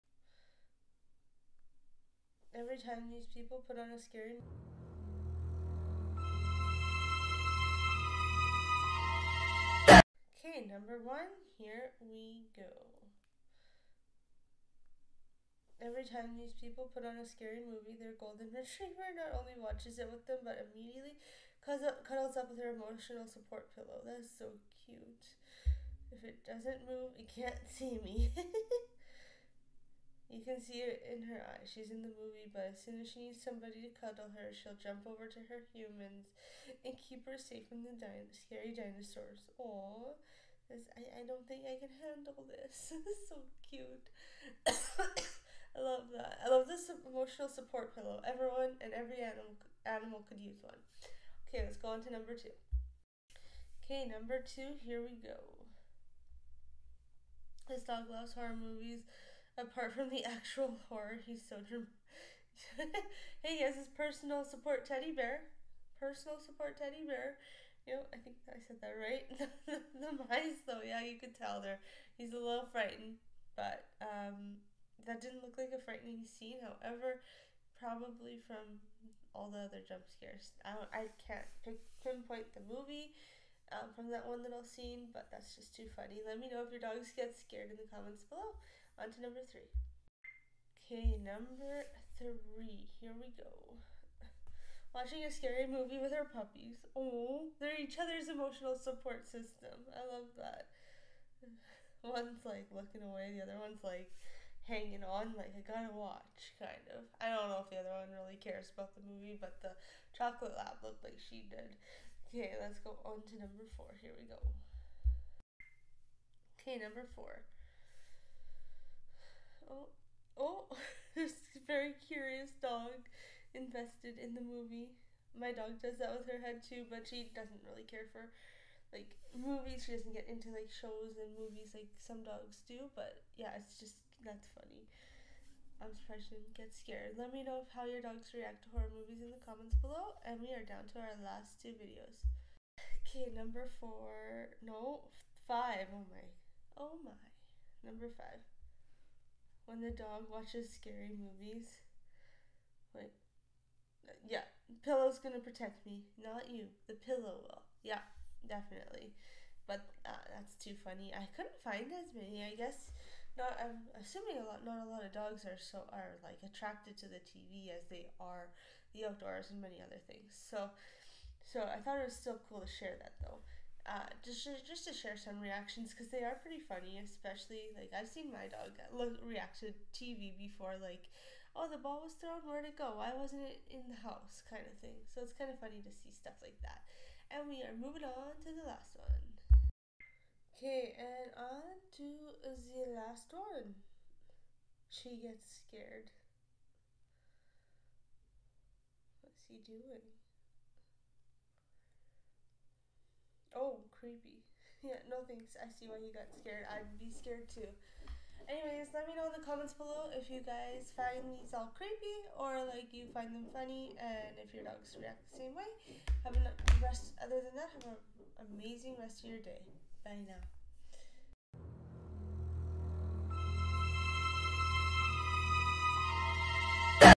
Funny Dogs Reacting To SCARY Movies!
Get ready for a howling good time as we put these scaredy pups to the test! Watch as they react to the spookiest of movies, from creepy classics to modern masterpieces of horror. Their hilarious reactions will have you laughing out loud and feeling a little better about being scared of the dark yourself. From trembling tails to terrified tantrums, these funny dogs prove that even the toughest of pups can be scaredy cats when it comes to spooky movies. So grab some popcorn, settle in, and enjoy the paws-itively hilarious reactions of these scaredy pups!